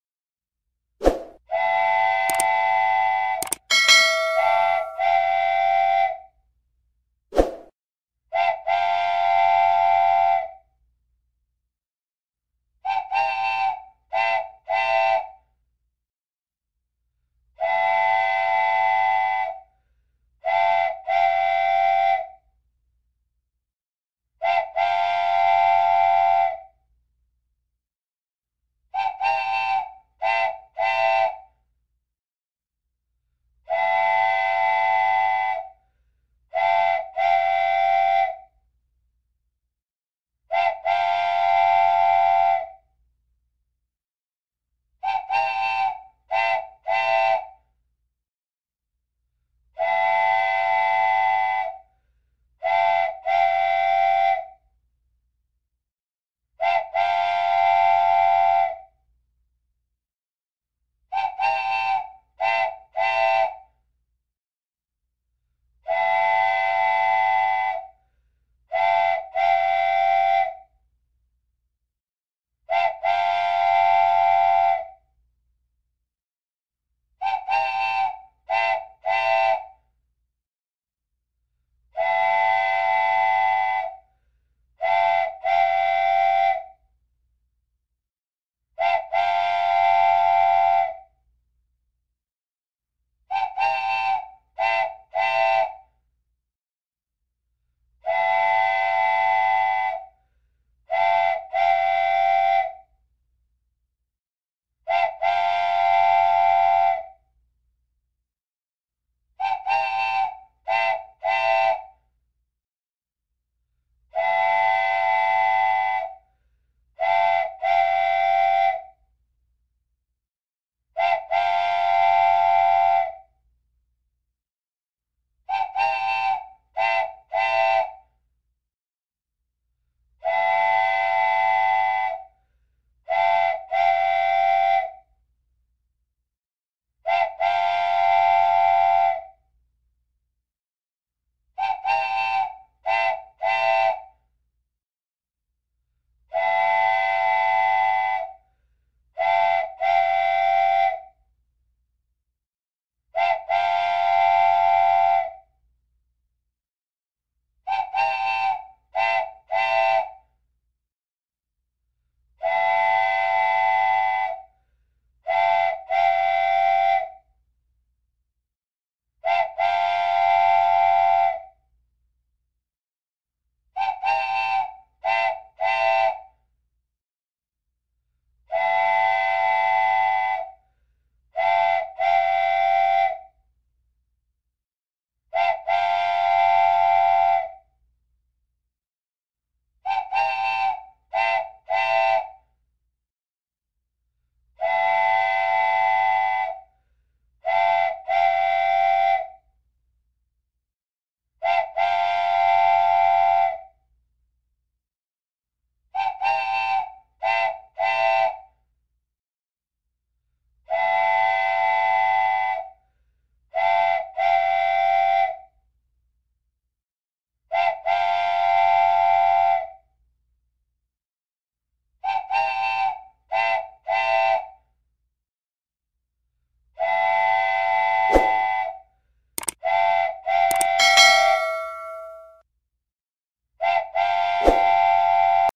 numbers counting| counting numbers for toddlers| number song|123 numbers | 1- 100
This video help kids to learn numbers from 1 to 10 with colorful images